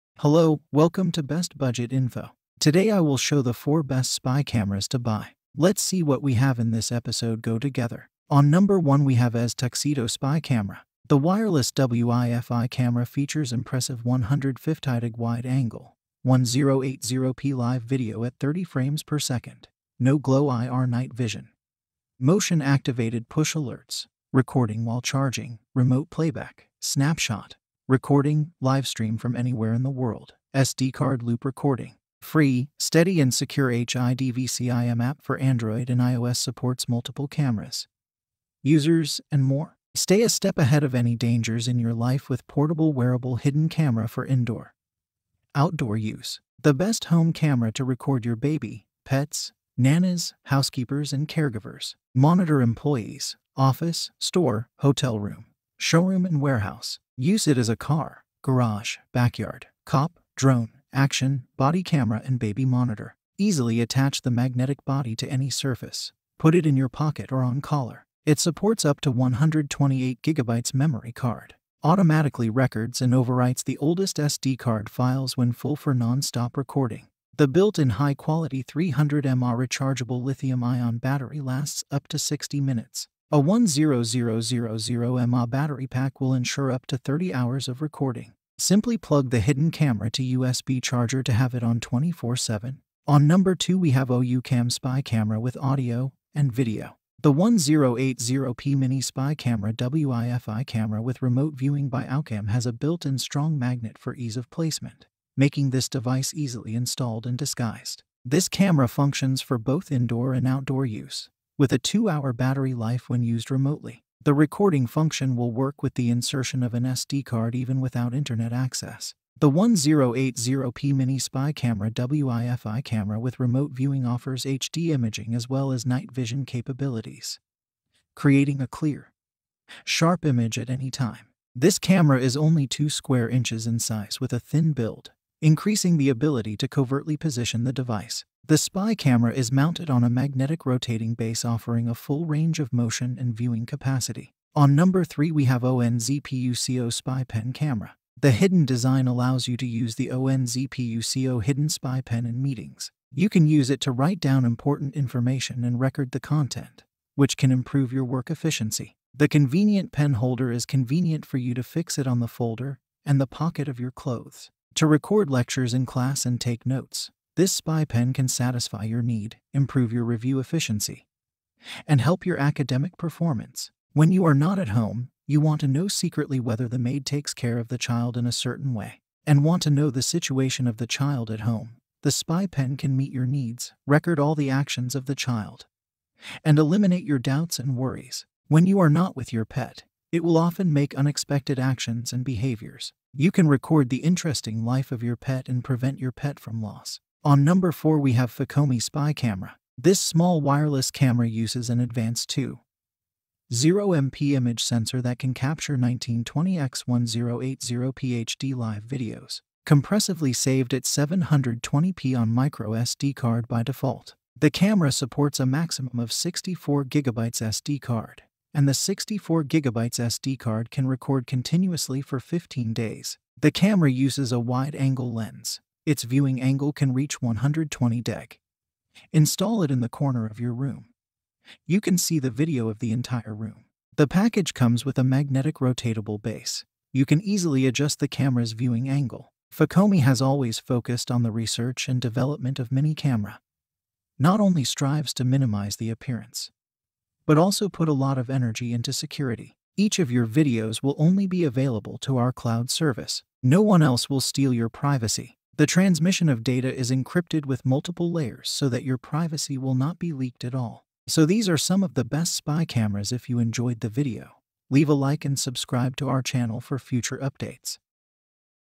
Spy Cameras with Audio and Video for home in 2023
Here are the 4 best useful wireless spy video cameras with Audio and video with the longest battery life to buy for home use and more.

best spy camera with longest battery life 2023
best easy to use spy camera
best spy camera with high resolution